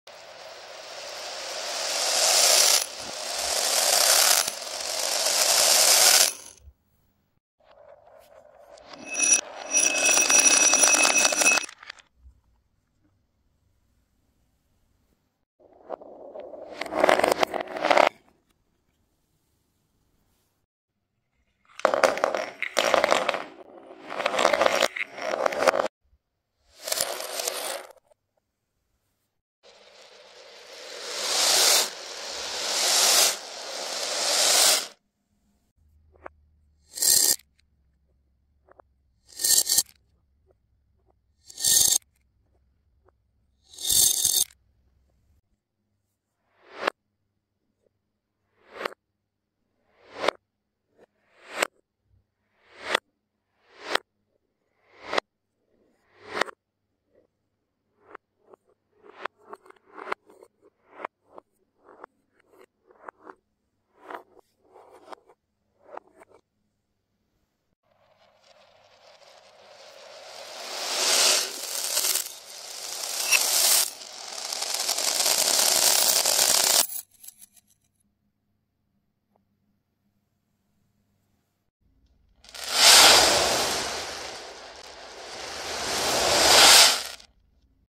Satisfying Video with Colorful Beads
In this video, satisfying video with colorful beads, you can see colorful beads, pearls, erasers, wooden cups and glass, all in reverse # satisfying reverse colorful asmr beads wooden color sound relaxing @worldinreverse